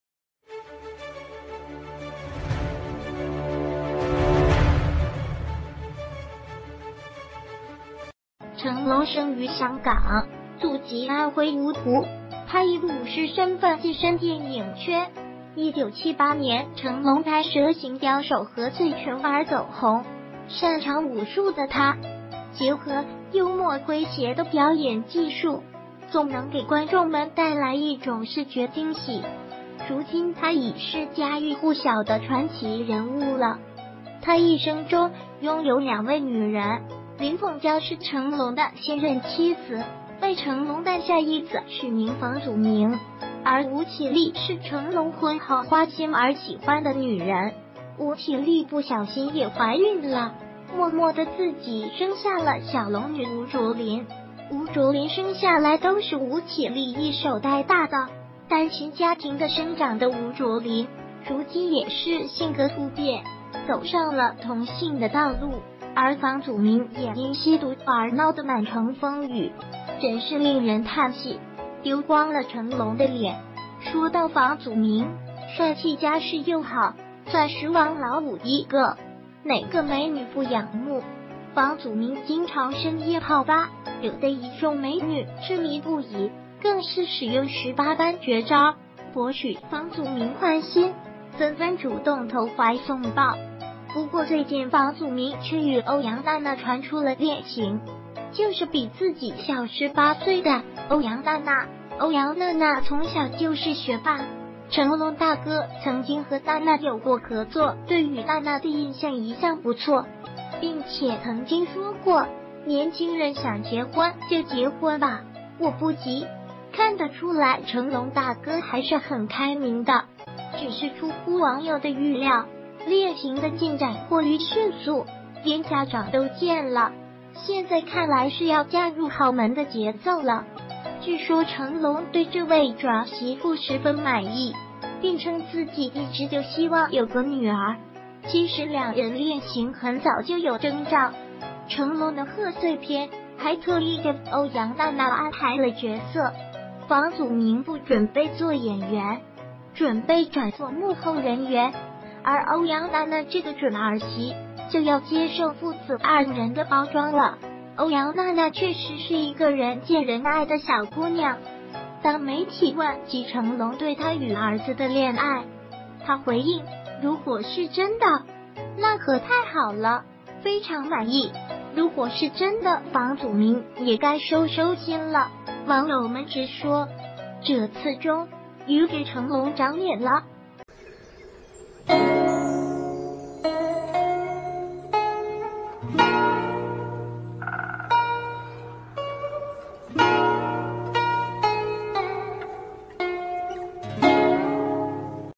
房祖名戀情曝光 成龍十分滿意終于長臉了
成龍生于香港，祖籍安...